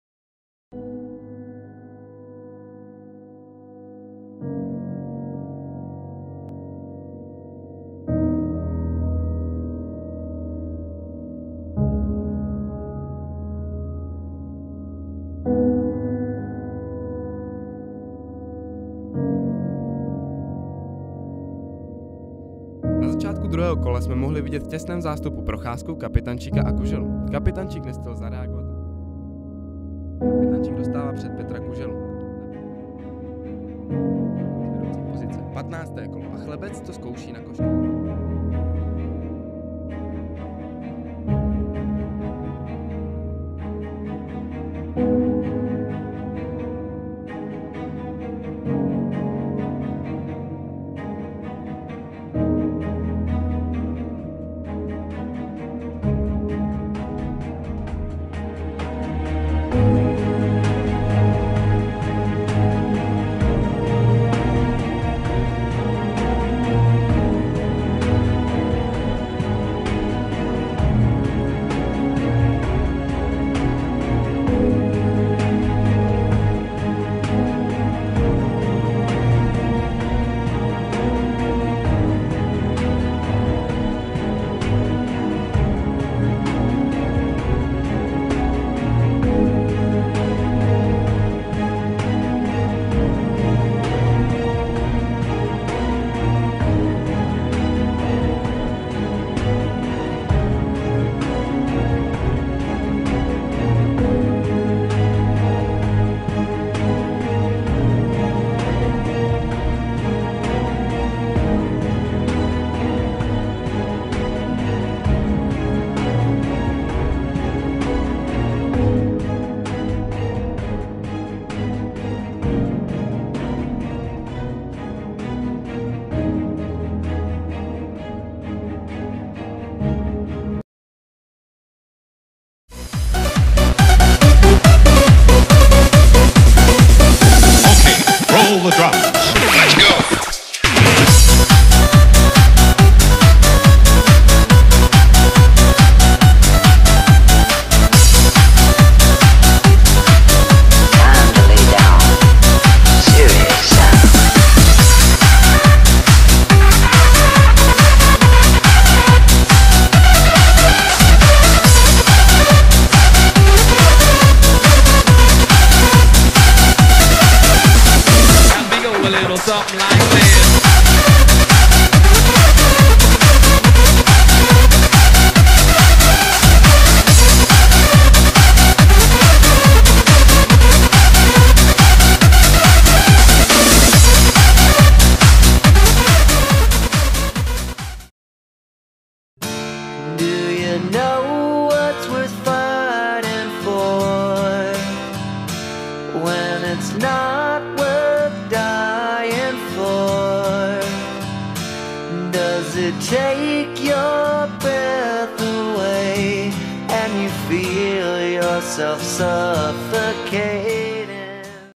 F1-liga - Trailer /Školní práce/
Školní projekt 2015.
Edit: Petr Bezdíček
Záběry ze hry: Petr Hejzlar